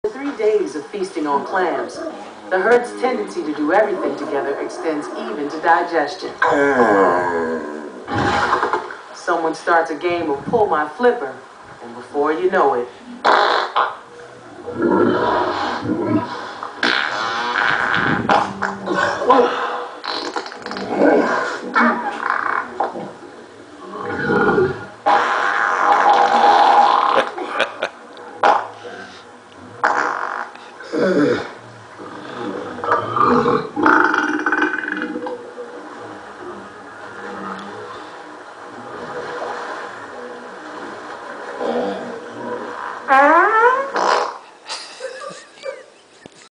Play A Game of Pull My Flipper
nothing says "FUN" better than a bunch of your walrus hommies and chics laying around after feasting on a bunch of clam and someone decides to pull out the old walrus game called "Pull My Flipper".

Next thing you know, people are turning their heads, blinking their eyes and even covering up their faces in pure discuss.  Genius